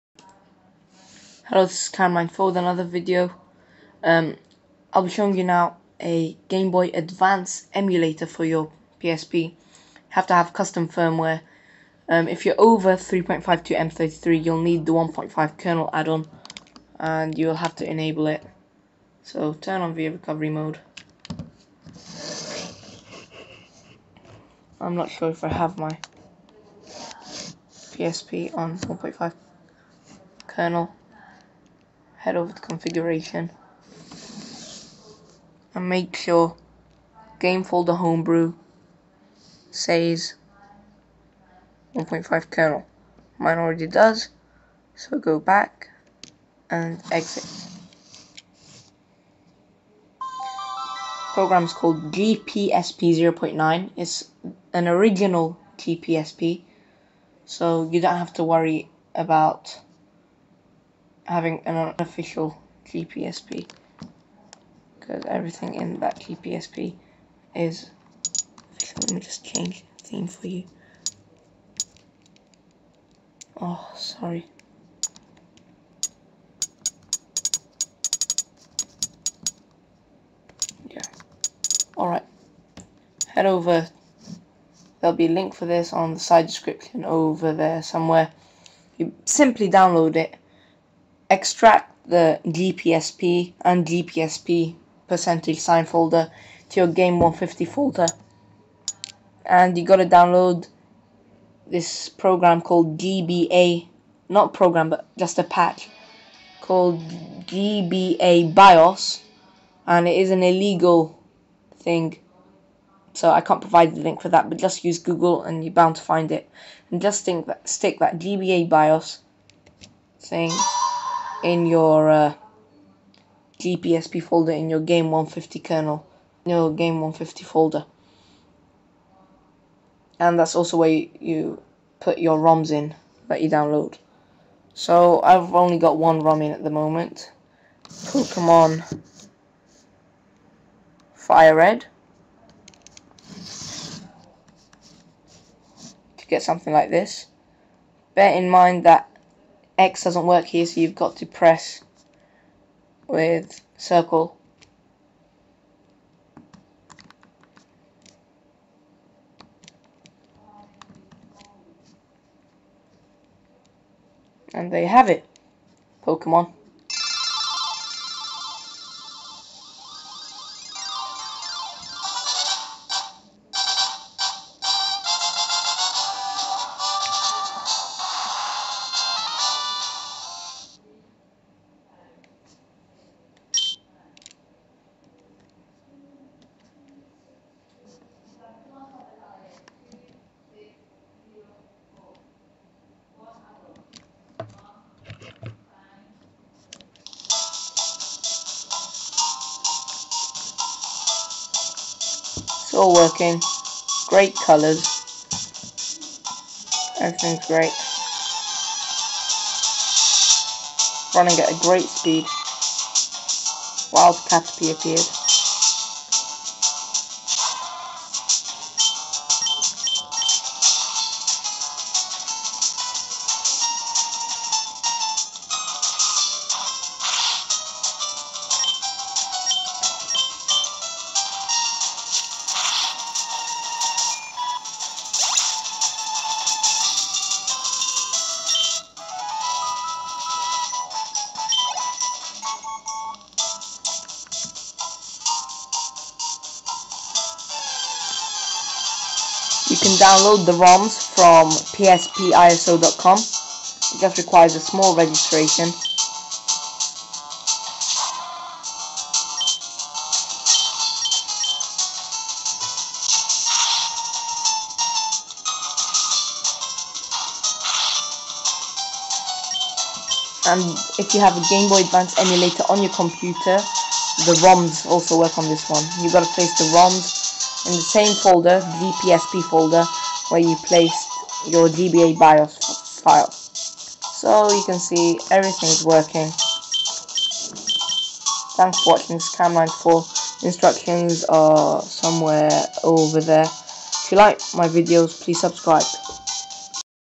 GBA Emulator on PSP
Download GBA-BIOS File:
USE GOOGLE.

Instructions:
Have 1.50 Kernal ENABLED.
Download, Extract both Folders:
GPSP & GPSP% To your GAME150 Folders.
Put your ROMS and GBA BIOS in your GPSP Folder.
Go to GAME on your PSP and Play GPSP.